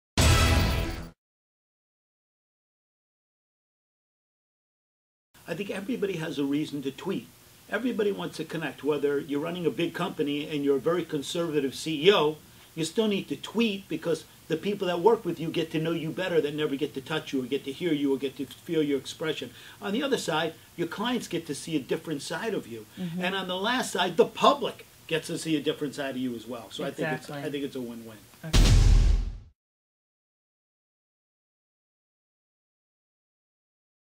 Should A CEO Or Business Owner Use Twitter
| Should CEO's and business owners use Twitter? Unbelievably, some people are not seeing the benefit of Twitter. My answer? A resounding YES!

By using Twitter, CEOs can connect on a one-to-one level with clients, customers and the general public. That engagement is VERY valuable.